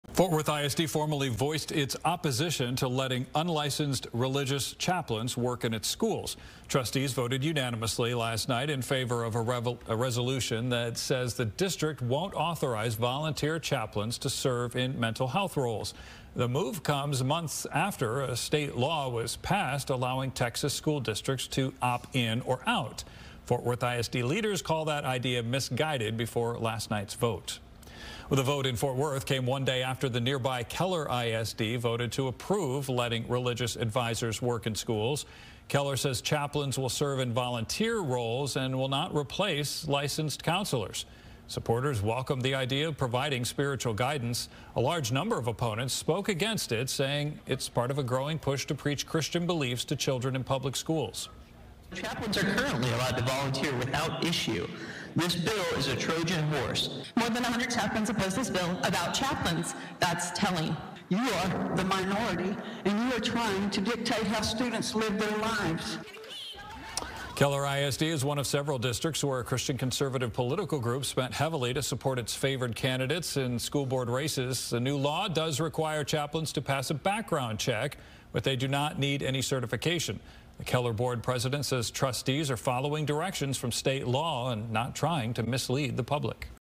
Fort Worth ISD won't let unlicensed chaplains in schools
A pair of North Texas school districts took opposing views on bringing volunteer, faith-based counselors onto their campuses. Fort Worth ISD rejected the idea while Keller ISD embraced it.